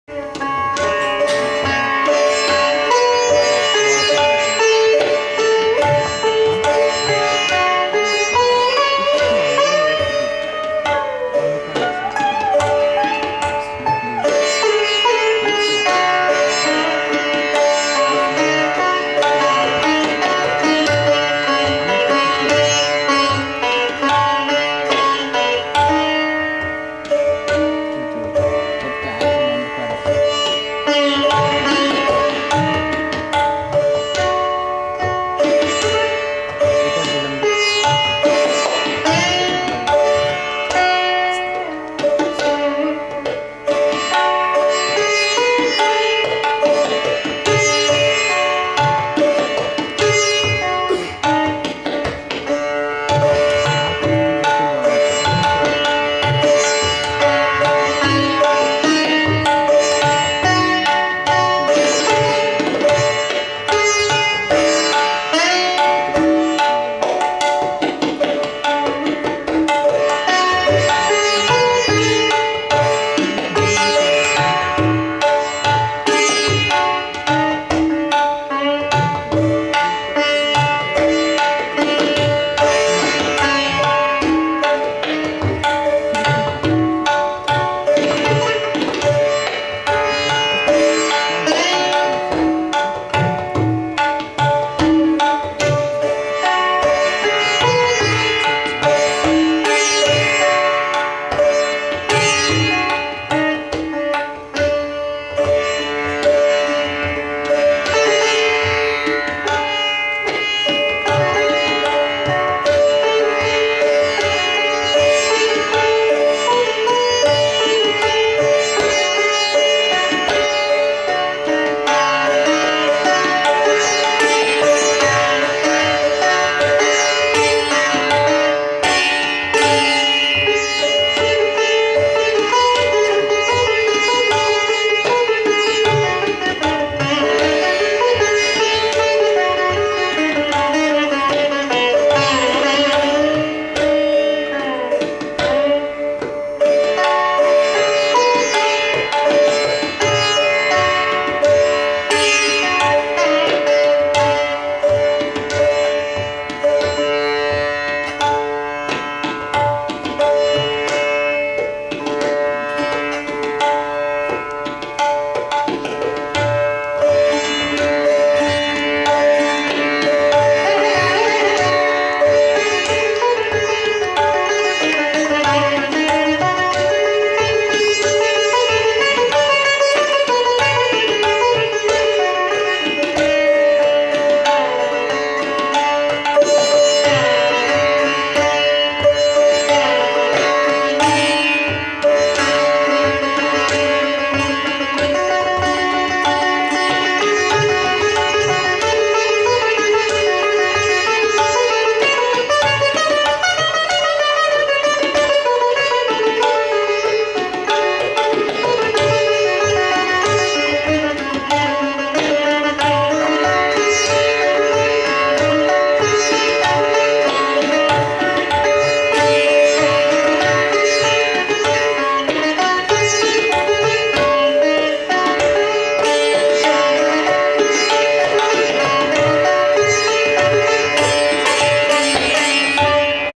Purya kalyan Vilambit
USC Concert by renowned Sitar Maestro Aloke Dasgupta

Purya Kalyan Ati Vilambit. UCSB Concert.

Concerts and Private Sitar instruction available